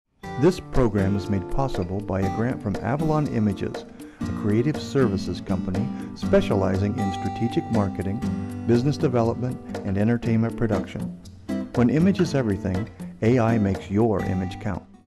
Sentinel Underwriter: Avalon Images (spot #1)
This is an acknowledgement to Avalon Images for their support of independent, grassroots local media.

This program is made possible by a grant from Avalon Images, a creative services company specializing in strategic marketing, business development and entertainment production. When image is everything, AI makes your image count.

This underwriting spot was produced by Charles Douglas in association with the Humboldt Sentinel.

Narration by Bob Young of Avalon Images.